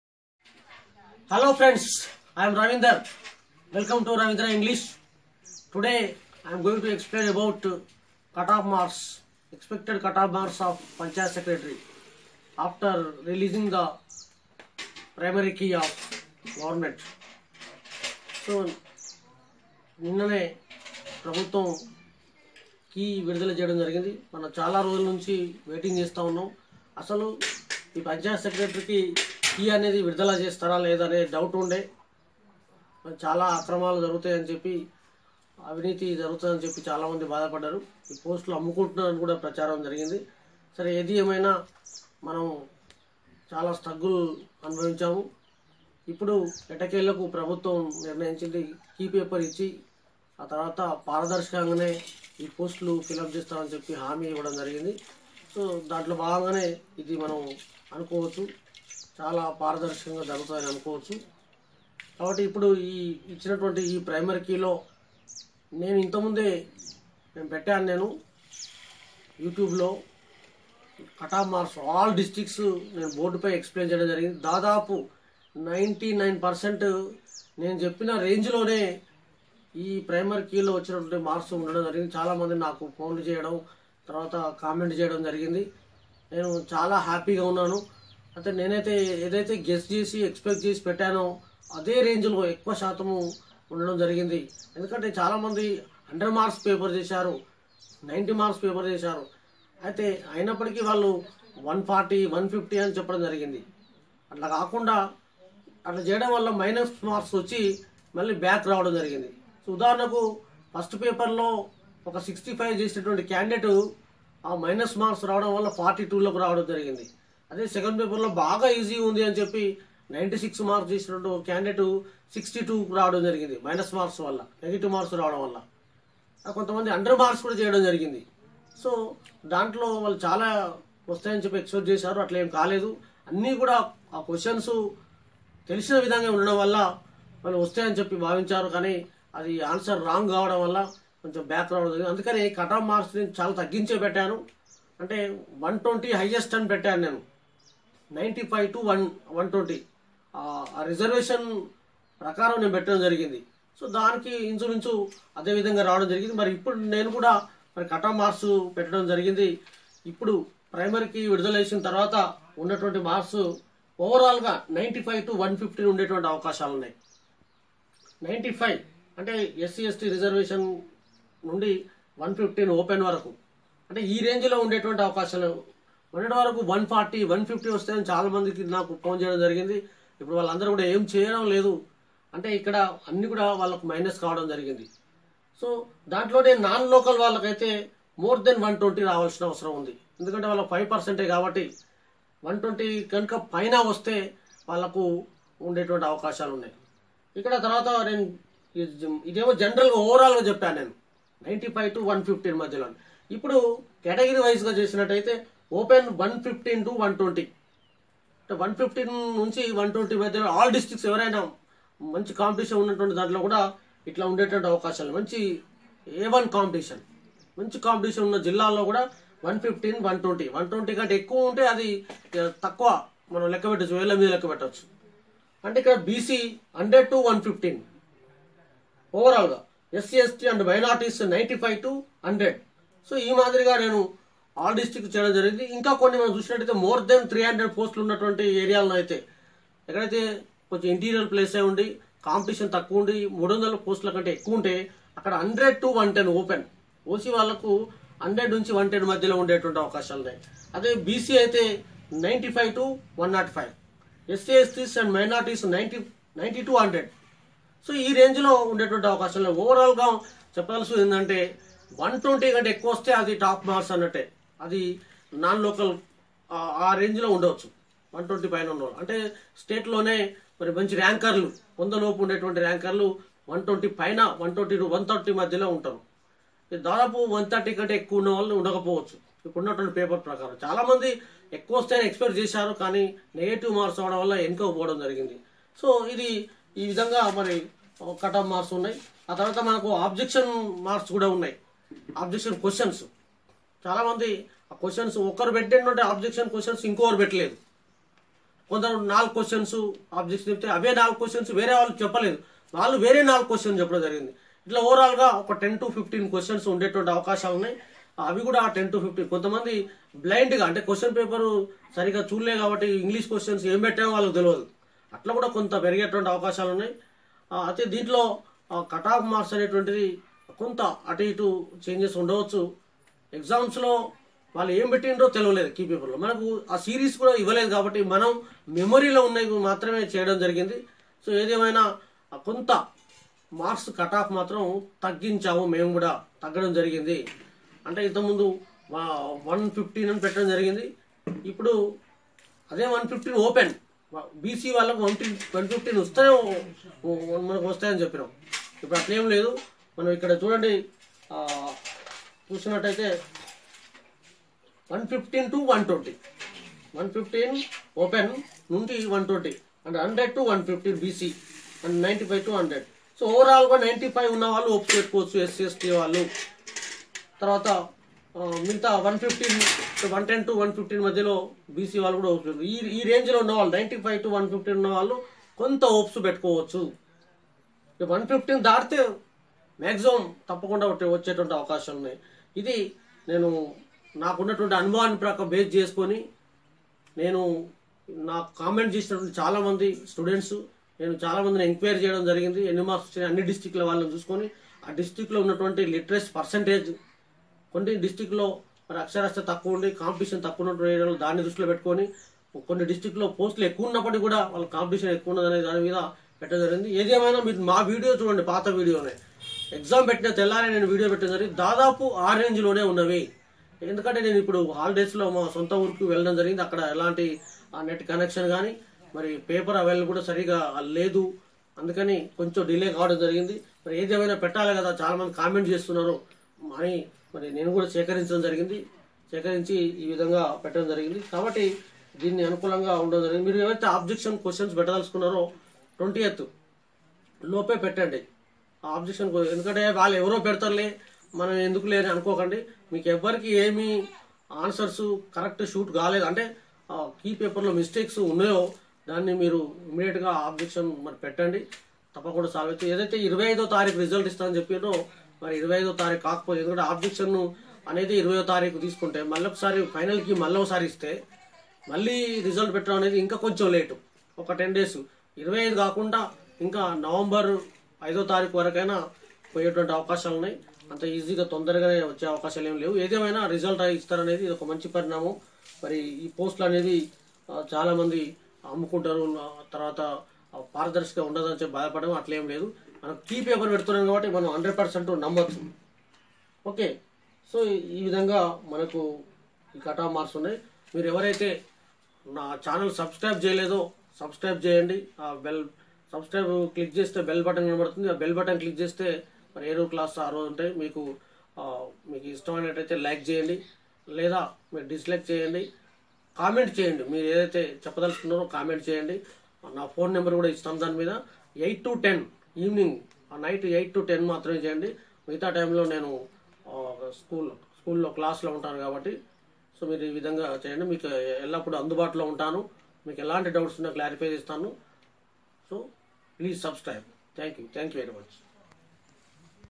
Expected Cut Off Marks of panchayat Secretary Exam after preliminary key.
I have explained about expected cut off marks of Panchayat secretary after government preliminary key.